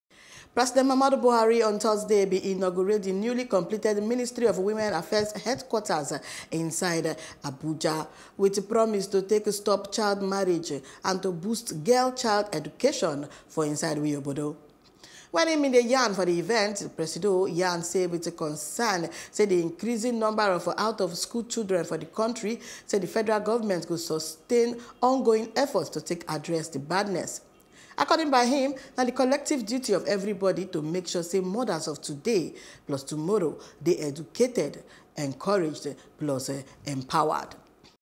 Buhari Don Inaugurate Women Affairs Ministry Building Inside Abuja
President Muhammadu Buhari on Thursday been inaugurated the newly completed Ministry of Women Affairs Headquarters Building inside  Abuja with a promise to stop child marriage and boost girl-child education for the country.